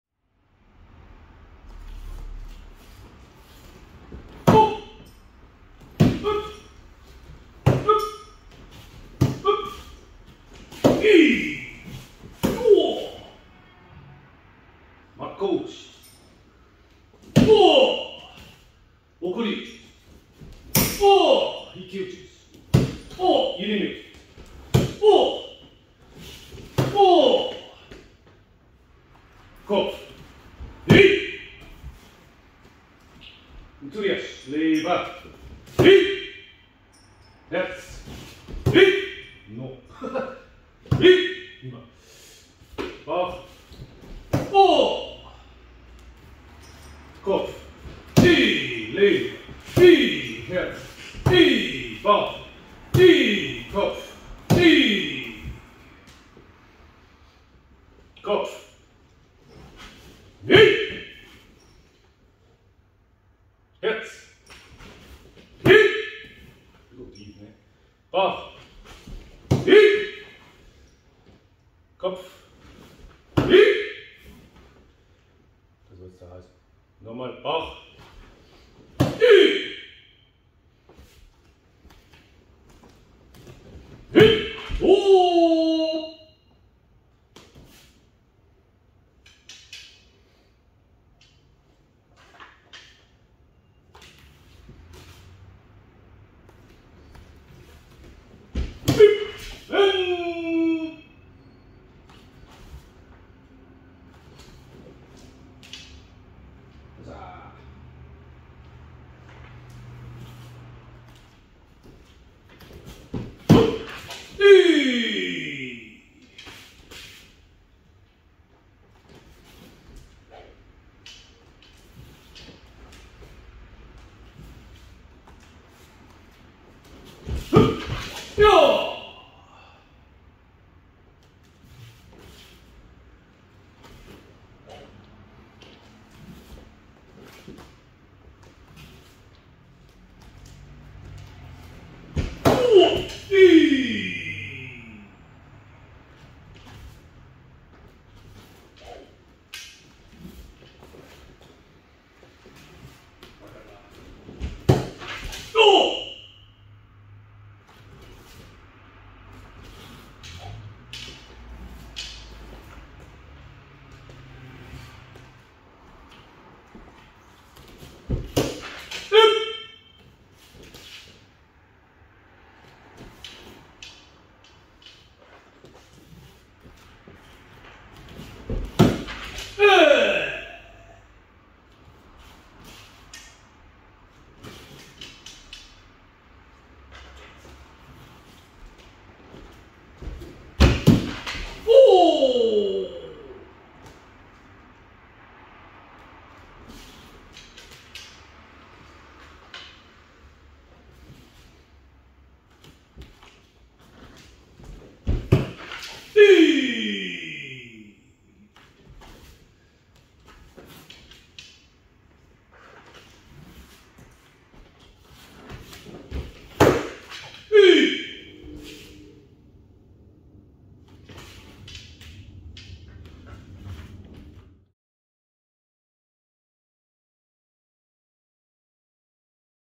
Budo Institut - Shurikenjutsu Training Japan 2023
a glimpse into the training of shurikenjutsu takeda ryu in Japan 2023